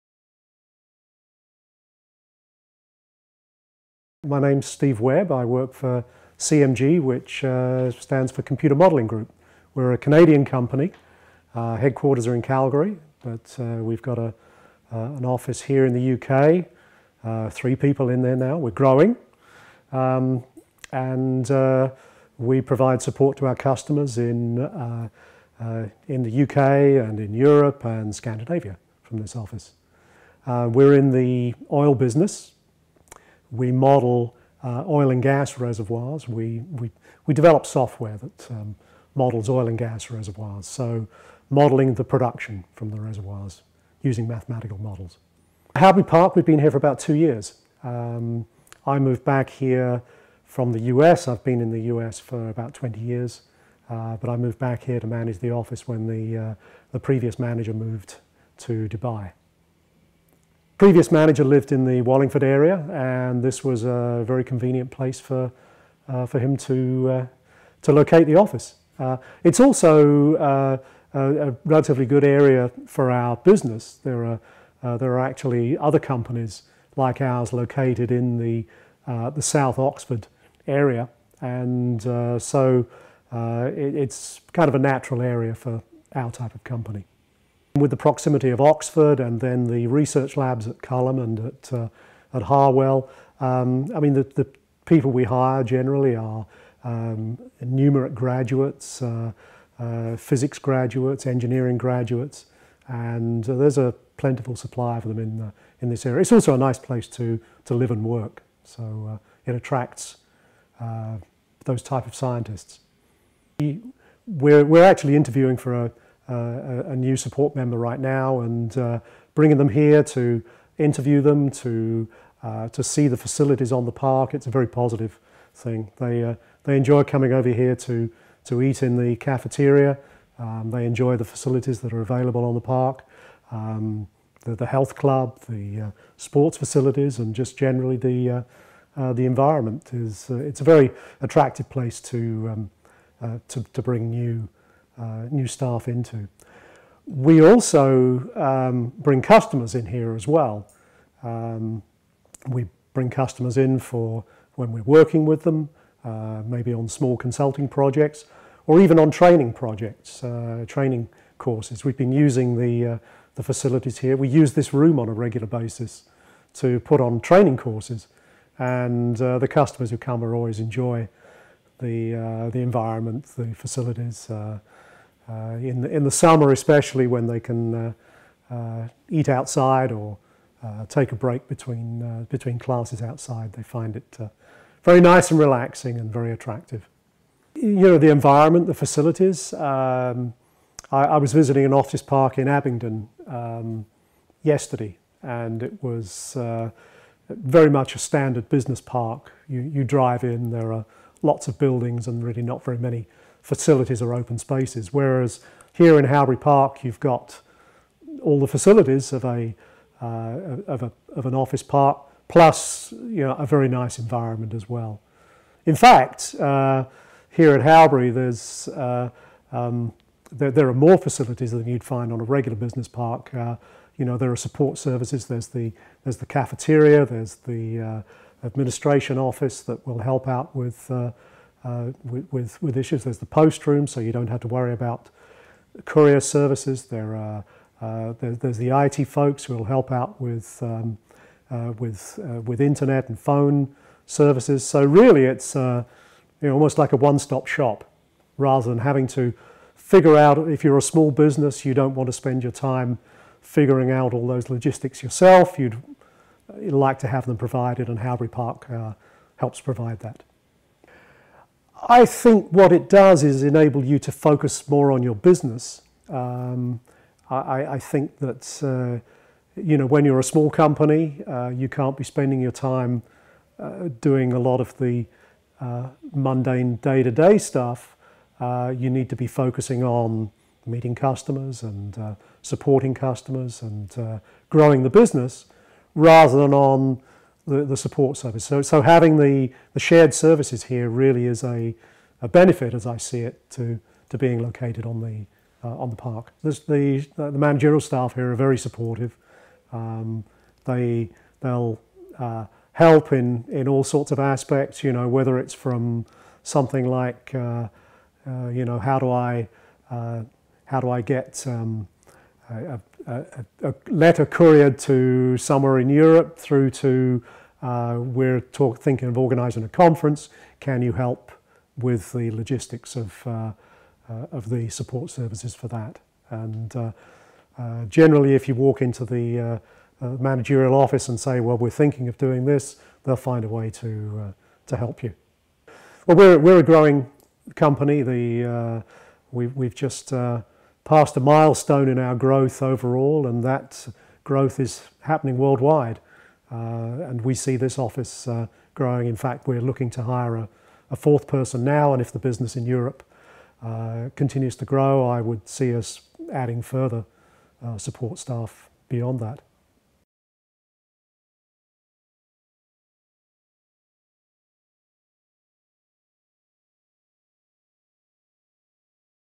Steve Webb, CMG, talks about local recruitment and amenities at Howbery Business Park
Steve talks about being based at Howbery Business Park, Wallingford, Oxfordshire. He highlights the difference and quality not normally found on more traditional business parks. He points out how the on-site amenities, especially the conference and meeting facilities have been a real bonus to his business.